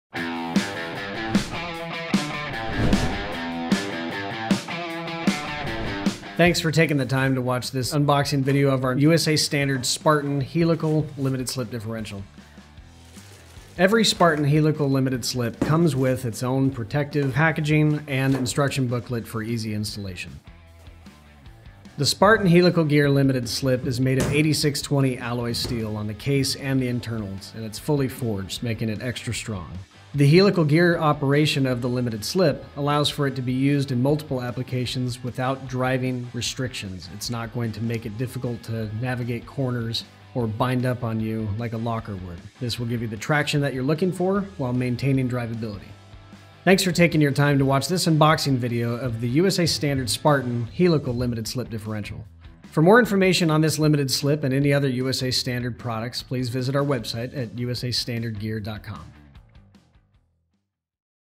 Unboxing the Spartan Helical LSD
Our team unboxes the newest member to the USA Standard list of products, the Spartan Helical LSD.  Compared to other types of limited slip differentials or positraction units, the Spartan Helical LSD provides smoother engagement, quieter operation, and requires no maintenance.